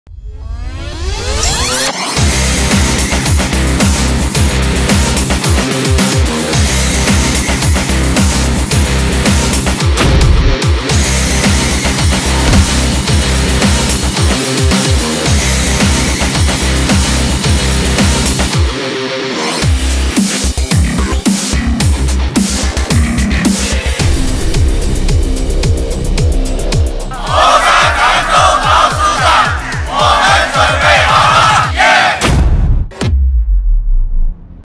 Warm and Professional: Moments from Our Exhibition Service
During the exhibition, our team showcased professionalism and enthusiasm, striving for excellence. From warm greetings to professional guidance, we ensure a hassle-free visitor experience. We strictly control every aspect, swiftly addressing any issues on-site to ensure the smooth operation of the exhibition.

Our services include:

Security Quality: Ensuring a safe and worry-free exhibition.
Sanitation Assurance: Maintaining cleanliness and health at the event.
Personalized Design: Creating a unique exhibition experience.
Every smile, greeting, and effort demonstrate our professional service. Through this behind-the-scenes video, we share the stories behind creating a perfect exhibition experience. We look forward to serving you again at future events, bringing even more excitement.